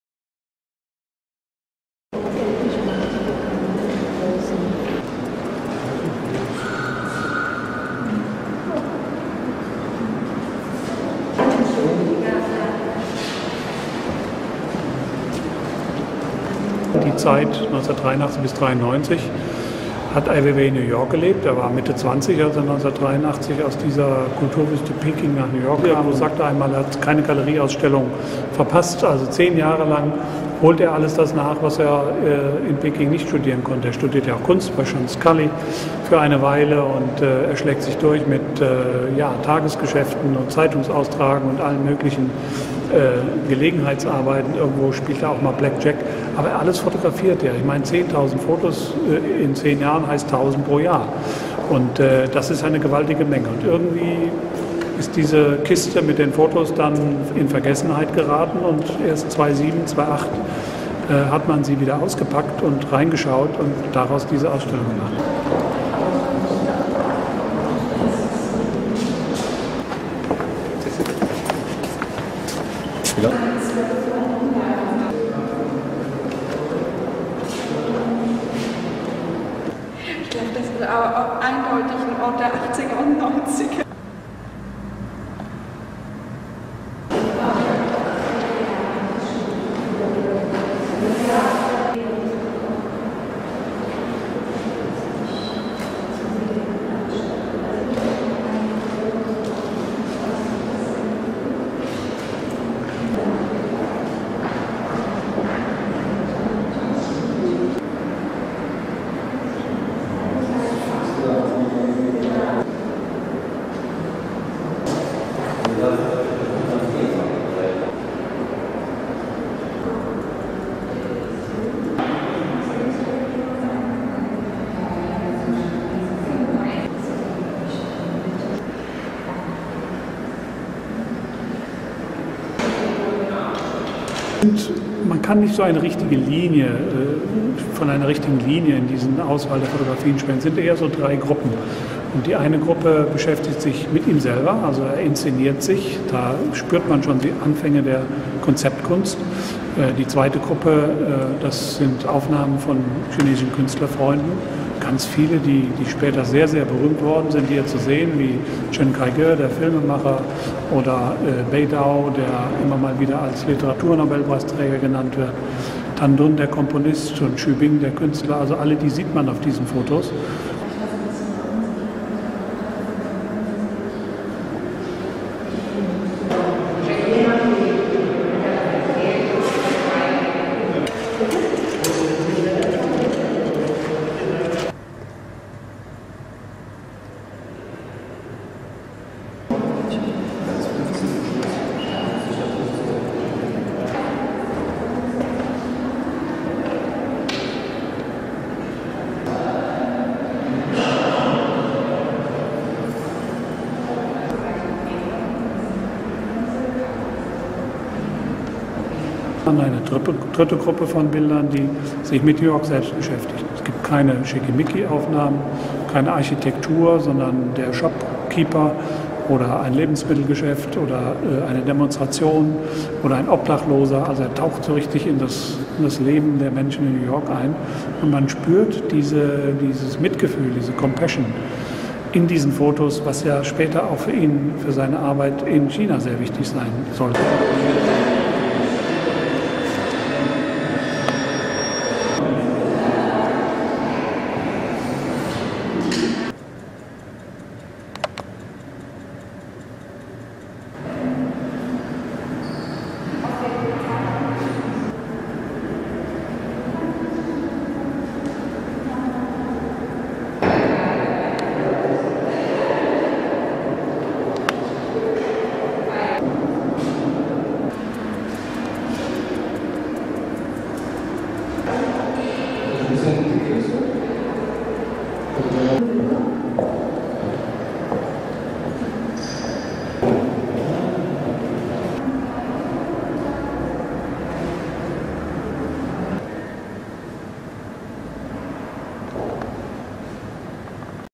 Ai Weiwei in New York - Fotografien 1983-1993
Interview (in German) with Gereon Sievernich, director of the Martin-Gropius-Bau, and impressions of the exhibition "Ai Weiwei in New York -- Fotografien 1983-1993" from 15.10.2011 to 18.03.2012 at the Martin-Gropius-Bau, Berlin... read more in German:

Interview mit Gropiusbau-Direktor Gereon Sievernich und Impressionen der Ausstellung "Ai Weiwei in New York -- Fotografien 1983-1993" vom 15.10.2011 bis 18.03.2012 im Martin-Gropius-Bau, Berlin.

Schnappschüsse eines Super-Stars: Chinas berühmtester Künstler lichtete als junger Emigrant in den USA Freunde und Alltags-Szenen ab. Eine Auswahl zeigt der Martin-Gropius-Bau -- aussagekräftig, aber leblos inszeniert.